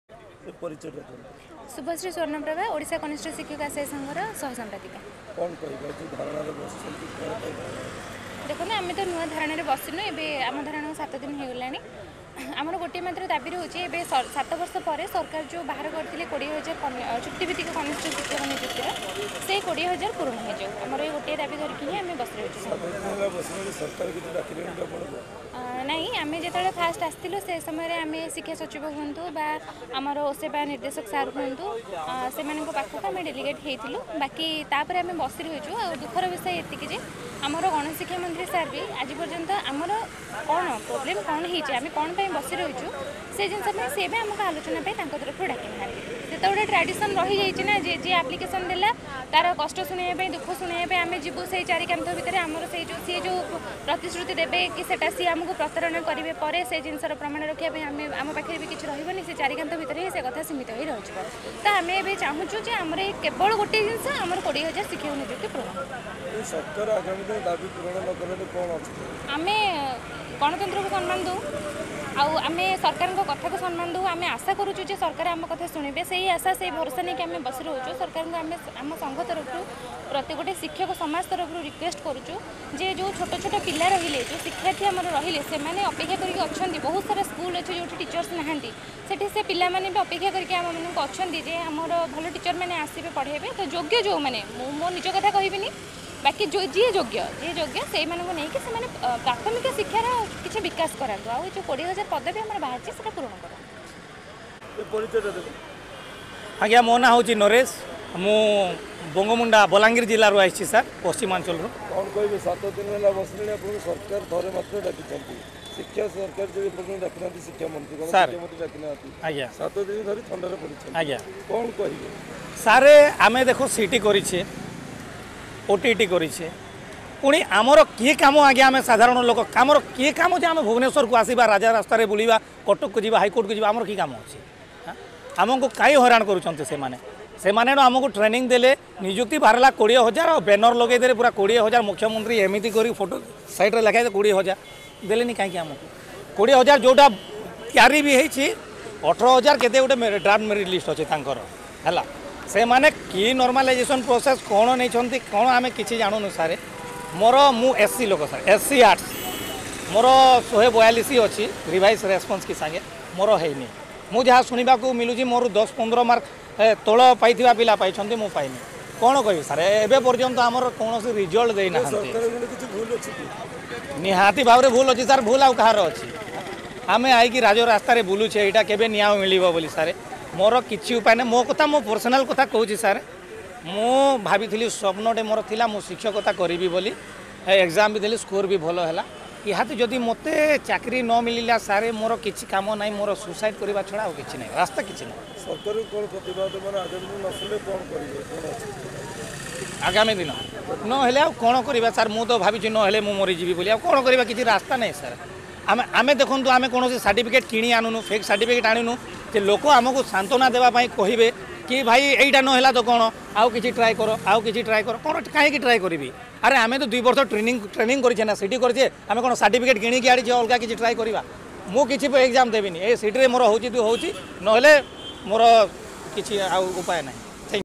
ଓଡ଼ିଶା କନିଷ୍ଠ ଆଶାୟୀ ସଙ୍ଘ ତରଫରୁ କୋଡିଏ ହଜାର ଶିକ୍ଷକ ନିଯୁକ୍ତି ଦାବିରେ ଗଣଧାରଣା | Times Odia | Bhubaneswar
ଓଡ଼ିଶା କନିଷ୍ଠ ଆଶାୟୀ ସଙ୍ଘ ତରଫରୁ କୋଡିଏ ହଜାର ଶିକ୍ଷକ ନିଯୁକ୍ତି ଦାବିରେ ଗଣଧାରଣା